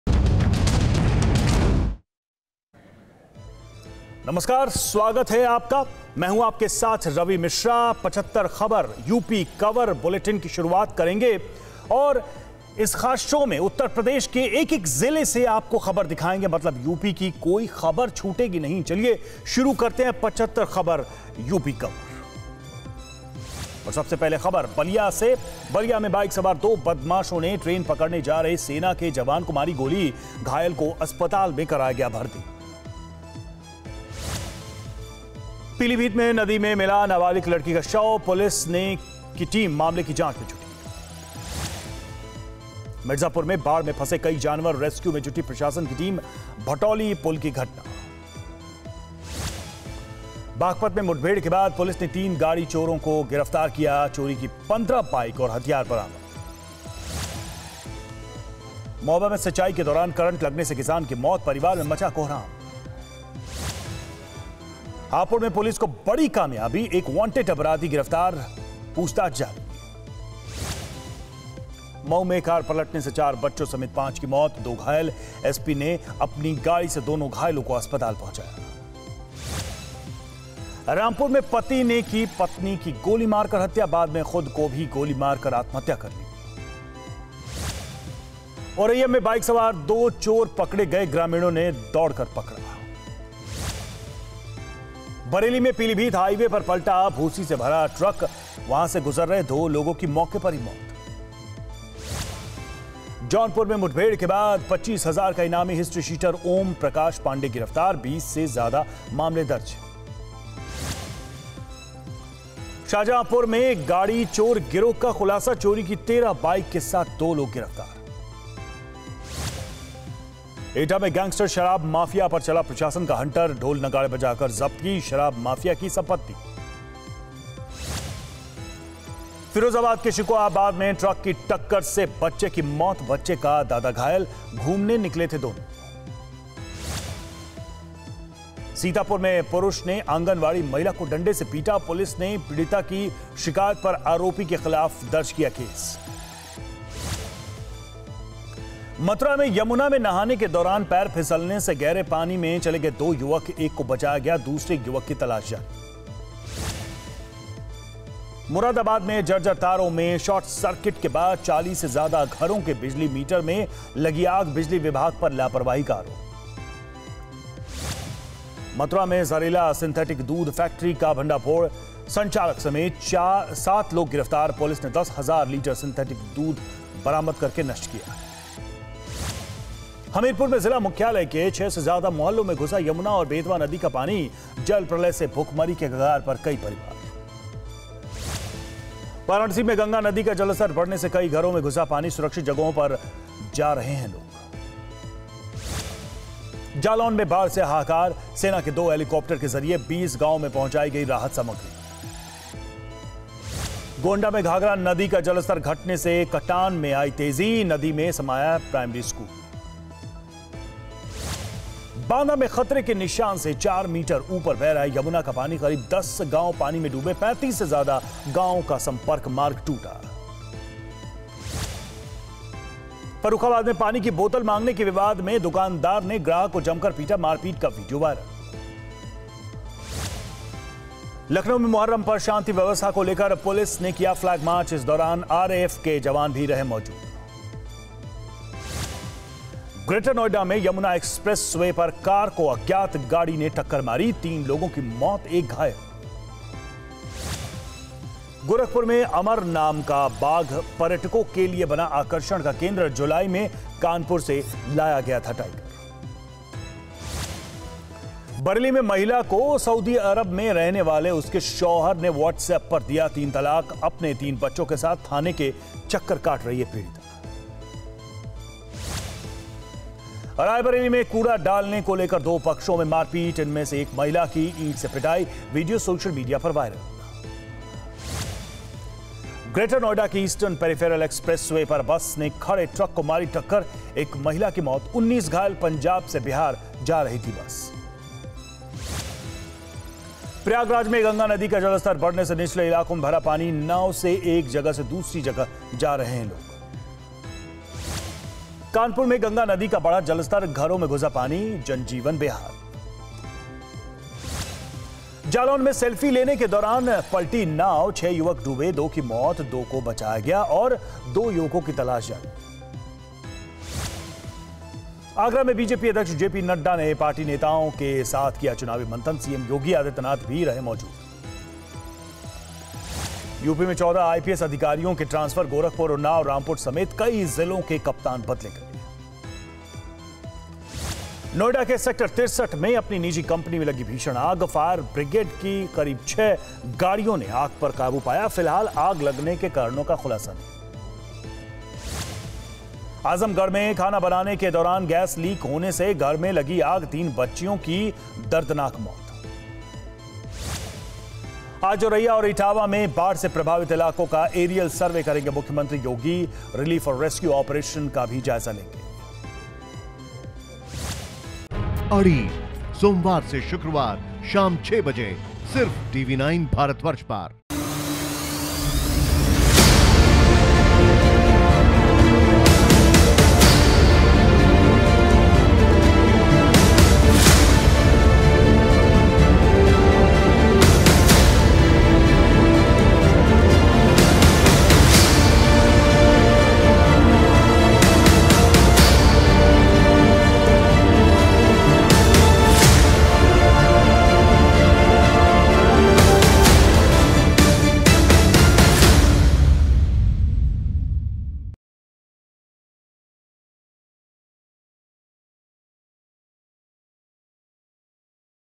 75 खबर UP कवर : Bareilly में Whatsapp पर दिया तीन तलाक
बरेली में महिला को सऊदी अरब में रहने वाले उसके शौहर ने व्हाट्सएप पर दिया तीन तलाक, पीड़िता अपने 3 बच्चों के साथ थाने के चक्कर काट रही है.

Credit